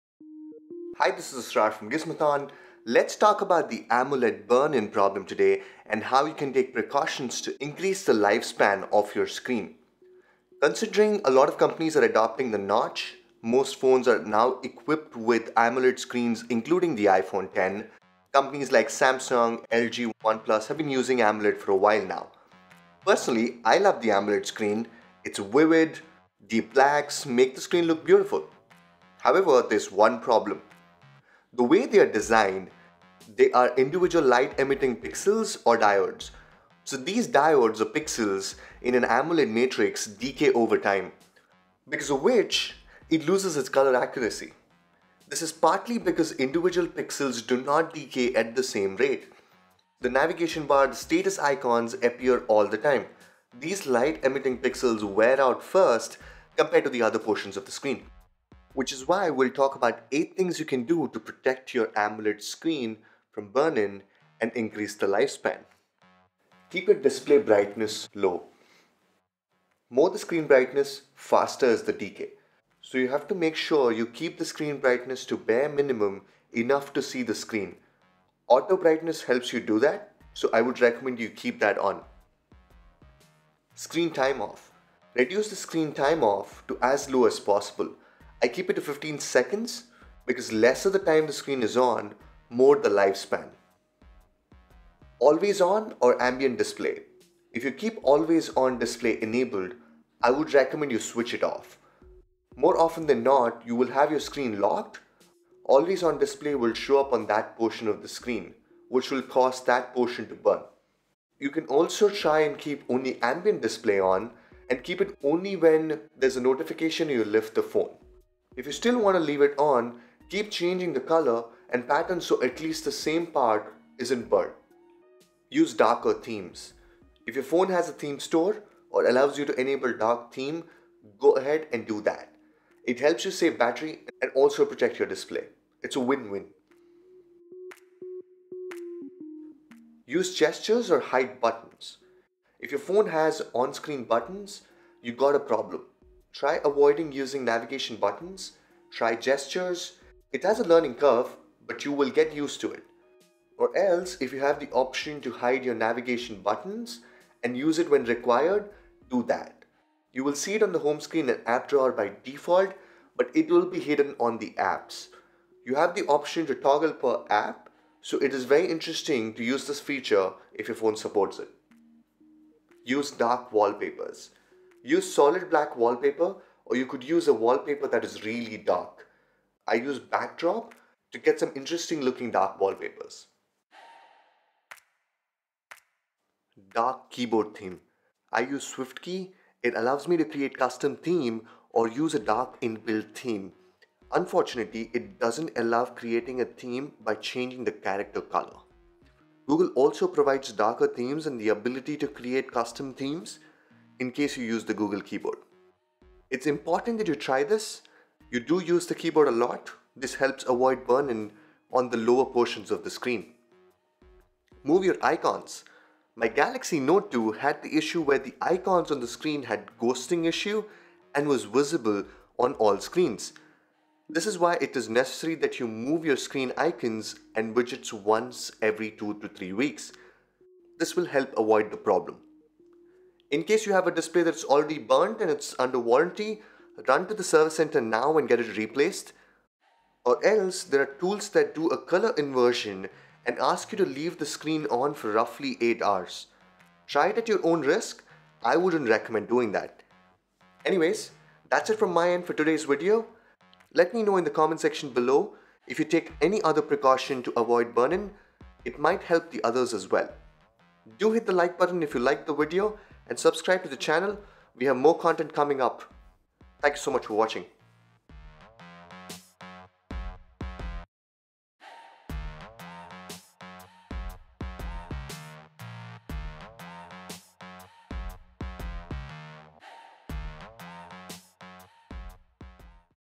8 Tips To Protect Your AMOLED Screen From Burn-In ft. OnePlus 6
AMOLED Screen Burn-In Problem.

1. Keep your display brightness low. More the screen brightness, faster the decay. So you have to make sure you keep the screen brightness low.

2. Screen Time-Off. Reduce the screen off time to as low as possible, I keep it to 15 secs. Lesser the time the screen is on, more the lifespan.

3. Always-On/Ambient Display: If you keep always on display enabled, I would recommend you switch it off.

4. Use Darker Themes: If your phone has a theme store or allows you to enable dark theme, go ahead and do that. It helps you save battery and will also protect your display.

5. Gestures/Hide Buttons: If you phone has on screen button, you got a problem. Try avoiding using navigation buttons, try gestures, it has a learning curve but you will get used to it. Or if you have an option to hide your navigation buttons and use it when required, do that. You will see it on the home screen and app drawer by default, but it will be hidden on the apps. You have the option to toggle per app, so it is a really interesting feature to use if your phone supports it.

6. Use dark wallpapers: Use Solid Black wallpaper or you could use a wallpaper that is really dark. I use Backdrop app to get some interesting looking dark wallpapers.

7. Dark Keyboard theme. I use Swiftkey, it allows me to create custom theme or use a dark inbuilt theme. Unfortunately, it doesn't allow creating a theme by changing the character color. Google also provides darker themes and ability to create custom themes, if you use the google keyboard. Its important that you try this, you do use the keyboard a lot, this help avoid burn in on the lower portions of you screen.

8. Move your icons. My Galaxy Note 2 had the issue where the icons on the screen had ghosting issue and was visible. This is why it is necessary that you move your screen icons and widgets once every 2-3 weeks. This will help avoid the problem

A lot of devices like Apple iPhone, Samsung S and Note series, OnePlus devices use AMOLED screens today, and you have to follow these tricks to help avoid the display burn.